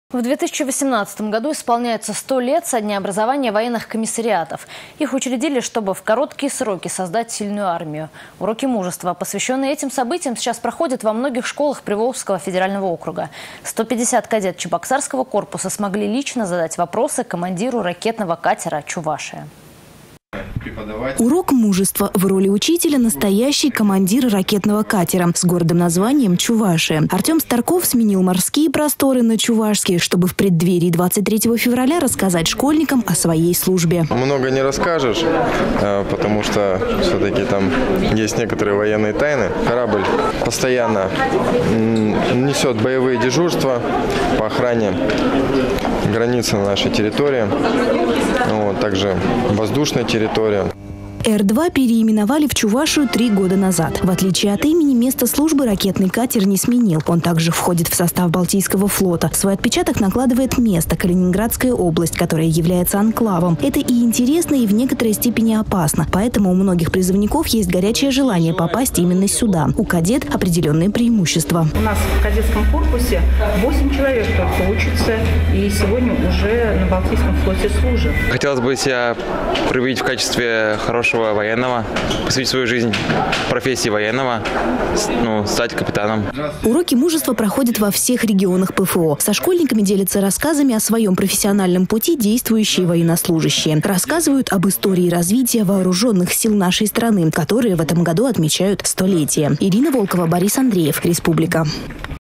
Уроки мужества с капитаном катера
В 2018 году исполнится 100 лет со дня образования военных комиссариатов. Их учредили, чтобы в короткие сроки создать сильную армию. Уроки мужества, посвященные этим событиям, сейчас проходят во многих школах Приволжского федерального округа. 150 кадет Чебоксарского корпуса  смогли лично задать вопросы командиру ракетного катера «Чувашия»